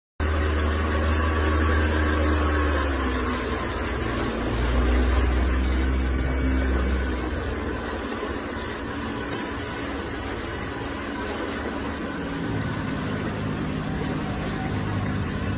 You won't Believe | hippopotamus Spotted In Bonny River Chasing A Speed Boat
You won't Be Believe | hippopotamus🦛
Spotted In Bonny River Chasing A Speed Boat 🚢

Hippos move through water at an average speed of about 5 mph (8 km/hr), but believe it or not, they can't really swim. Their bodies aren't adapted to paddling. Instead, hippos basically gallop along the river bottom.